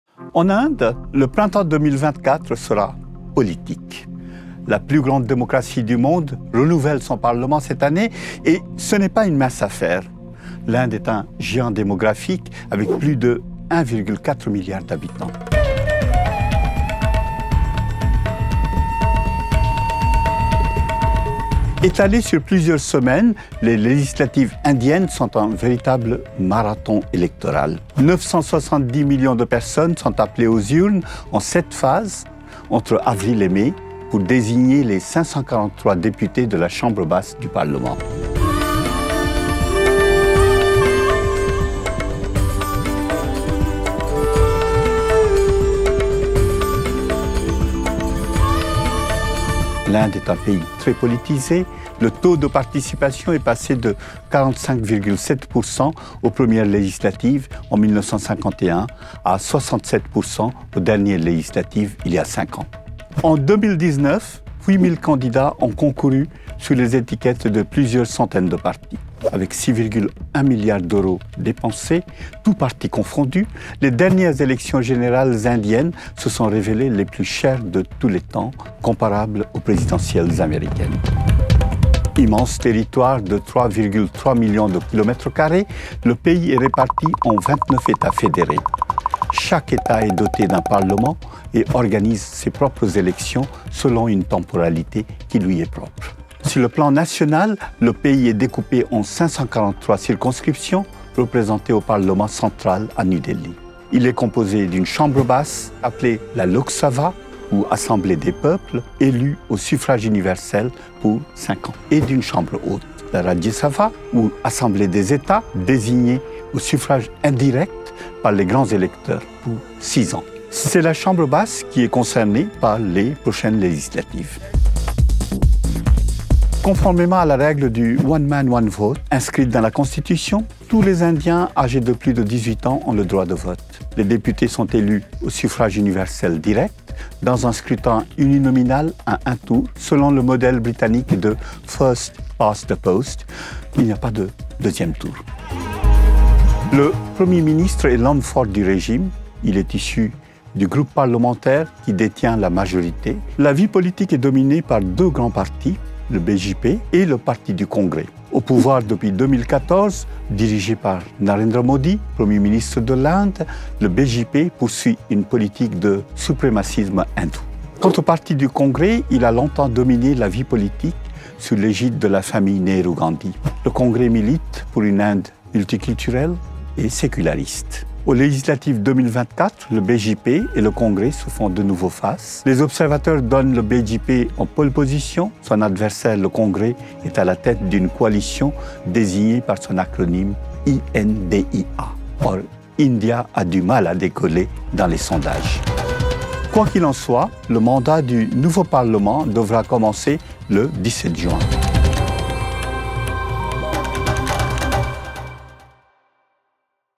Inde : élections, mode d'emploi • RFI
En Inde, ce printemps 2024 sera politique ! La plus grande démocratie du monde renouvelle la chambre basse de son Parlement : les législatives sont un marathon de sept semaines, au cours desquelles 970 millions de personnes sont appelées à élire 543 députés.